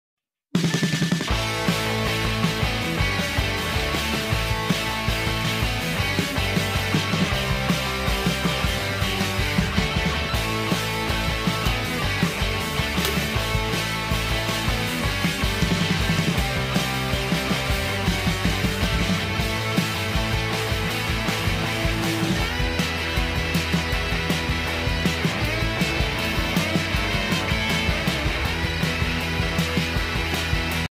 DESI JUGAAD To Pick Mango From Tree
Watch this desi jugaad to pick mango from tree. Mango will not fall on ground